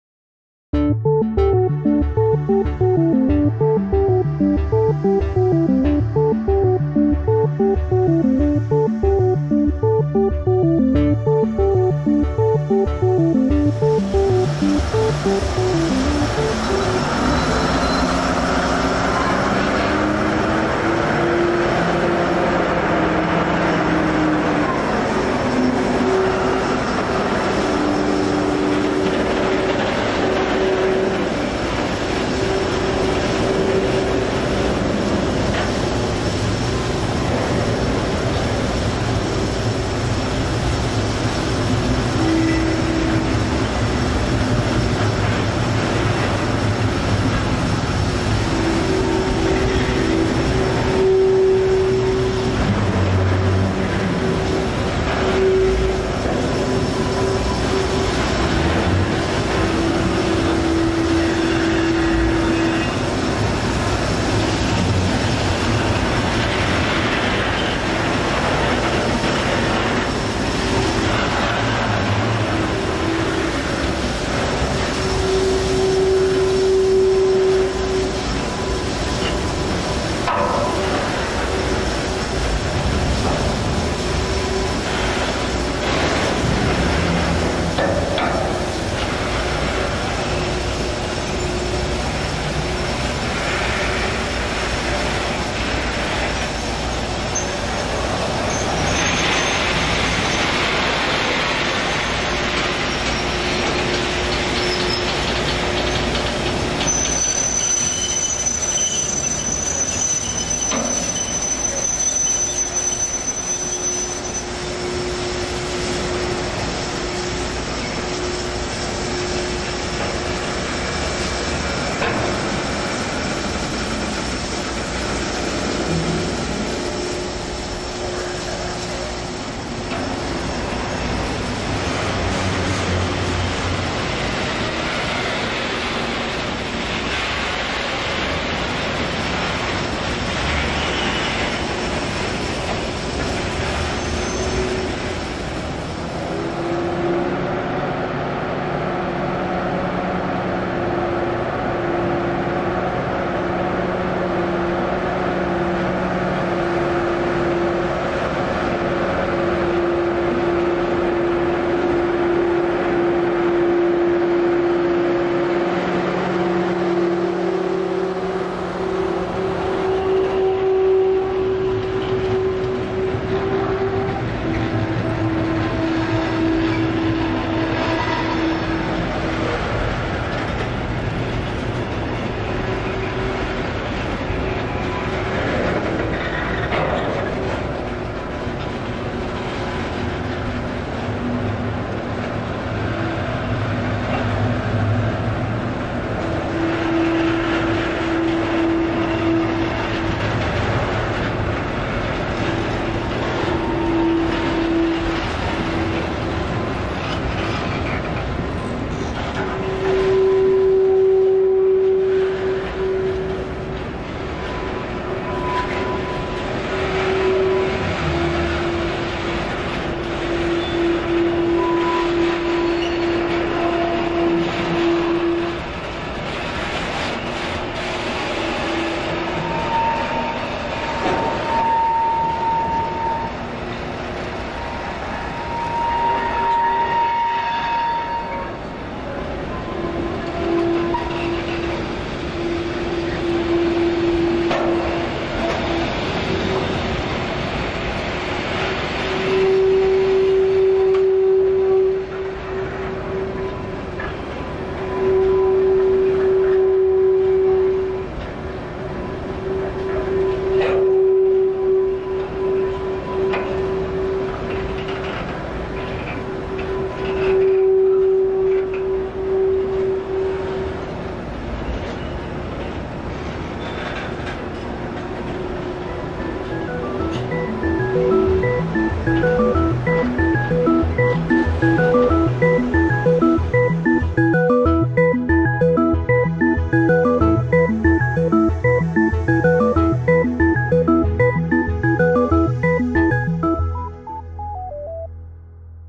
Rail Feeding bij Botlek Tank Terminal
RF 18 haalt een sleep lege ketelwagons uit bij de Botlek Tank Terminal aan de Chemieweg in Rotterdam-Botlek.